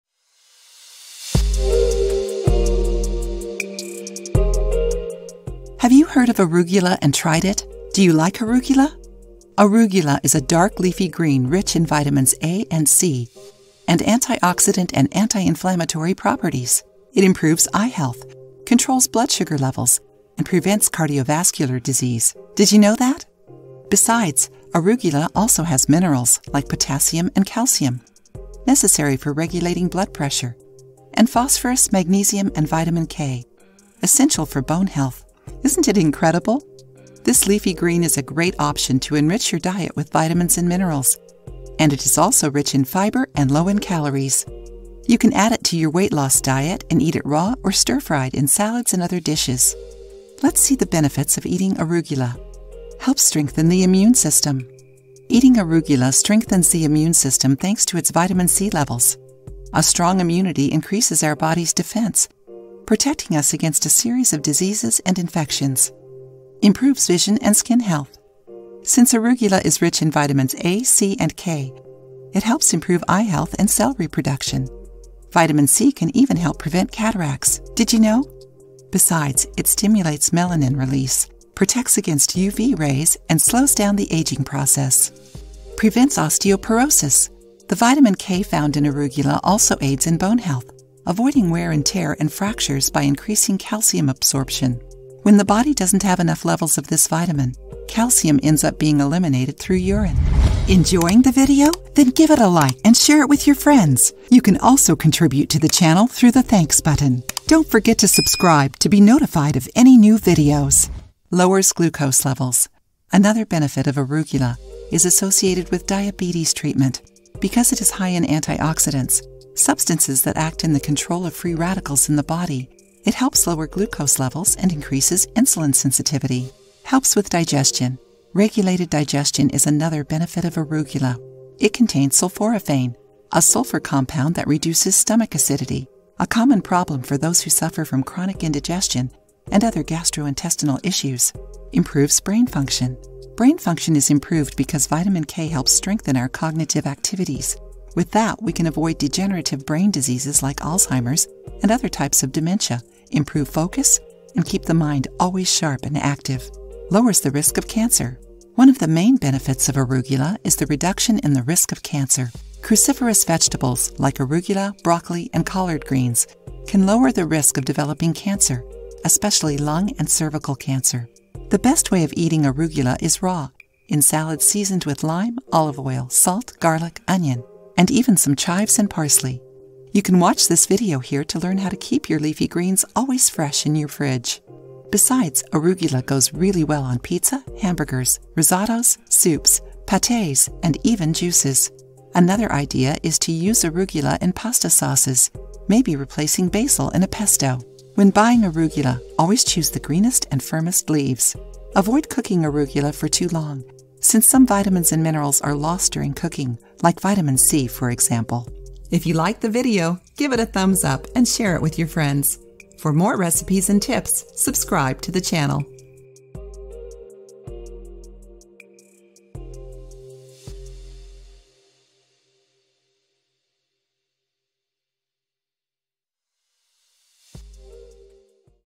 7 Reasons Why You Should Eat Arugula More Often
Arugula is a dark leafy green rich in vitamins A and C and antioxidant and anti-inflammatory properties.

It improves eye health, controls blood sugar levels, and prevents cardiovascular disease. Did you know that?

Besides, arugula also has minerals, like potassium and calcium, necessary for regulating blood pressure, and phosphorus, magnesium, and vitamin K, essential for bone health. Isn't it incredible?

This leafy green is a great option to enrich your diet with vitamins and minerals, and it is also rich in fiber and low in calories.

You can add it to your weight loss diet and eat it raw or stir-fried in salads or other dishes.

0:00 Health Benefits Arugula

0:52 Helps strengthen the immune system
Eating arugula strengthens the immune system thanks to its vitamin C levels.

1:06 Improves vision and skin health
Since arugula is rich in vitamins A, C, and K, it helps improve eye health and cell reproduction.

1:25 Prevents osteoporosis
The vitamin K found in arugula also aids in bone health, avoiding wear and tear and fractures by increasing calcium absorption.

1:50 Lowers glucose levels
Another benefit of arugula is associated with diabetes treatment.

2:06 Helps with digestion
Regulated digestion is another benefit of arugula.

2:21 Improves brain function
Brain function is improved because vitamin K helps strengthen our cognitive activities.

2:37 Lowers the risk of cancer
One of the main benefits of arugula is the reduction in the risk of cancer.

2:51 How to eat arugula
The best way of eating arugula is raw, in salads seasoned with lime, olive oil, salt, garlic, onion, and even some chives and parsley.

When buying arugula, always choose the greenest and firmest leaves.

Avoid cooking arugula for too long, since some vitamins and minerals are lost during cooking, like vitamin C, for example.